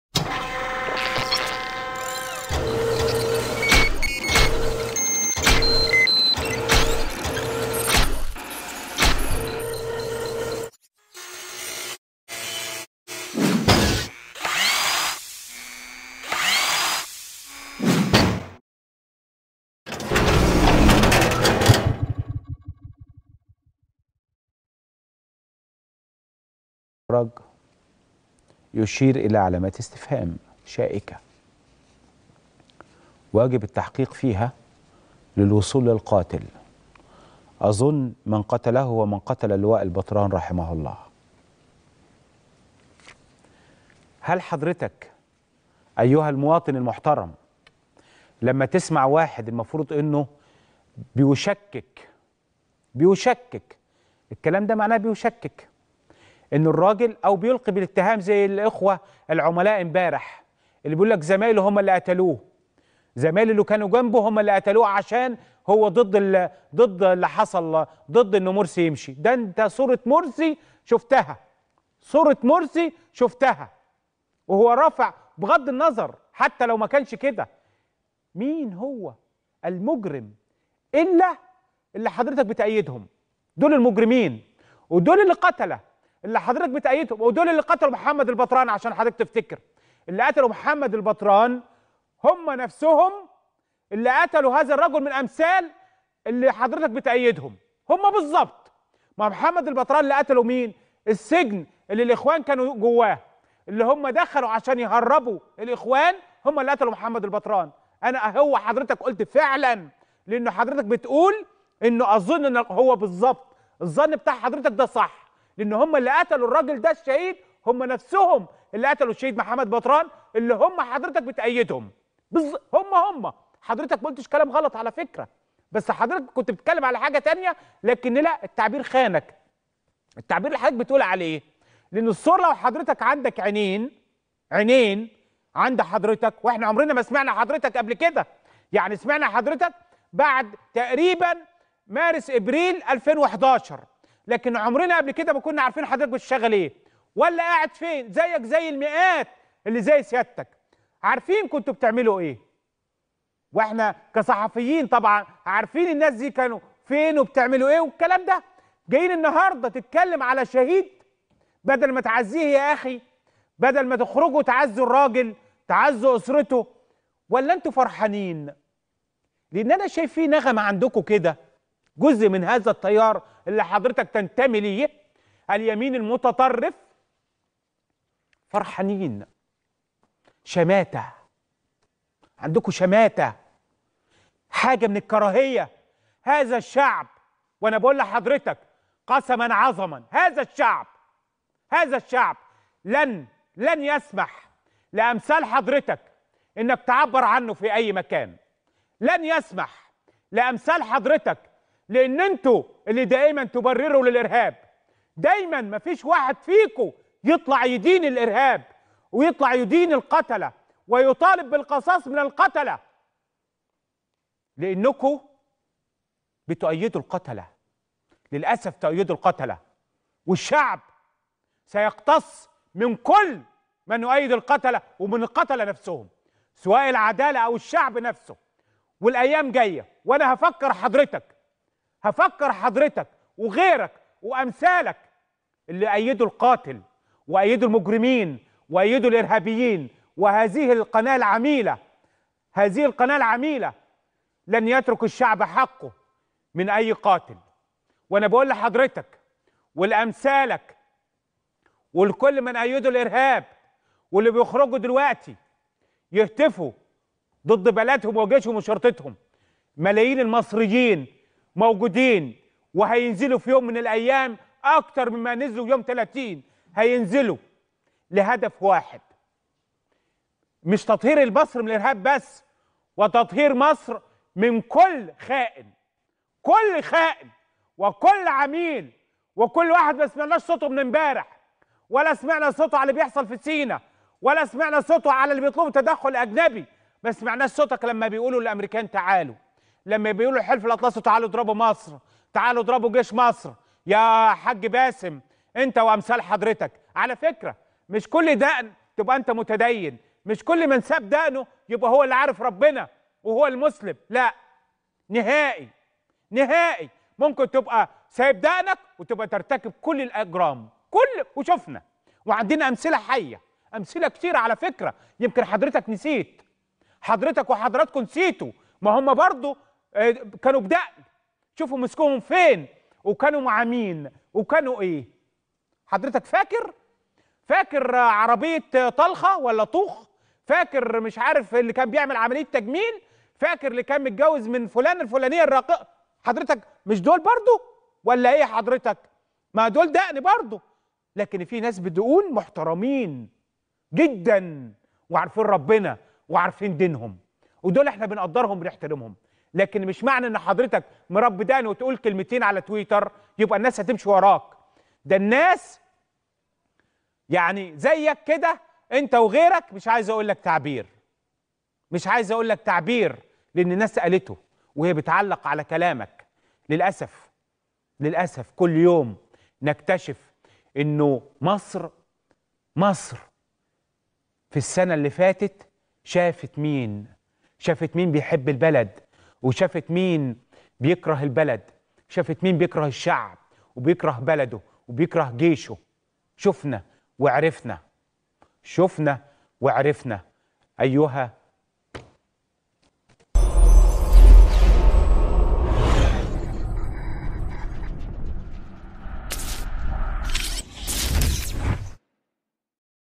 انفعال احمد موسى على شخص يشكك فى استشهاد اللواء نبيل عبد المنعم فراج
انفعال شديد احمد  موسى  على  شخص  يشكك   فى  استشهاد   اللواء  نبيل  عبد  المنعم  فراج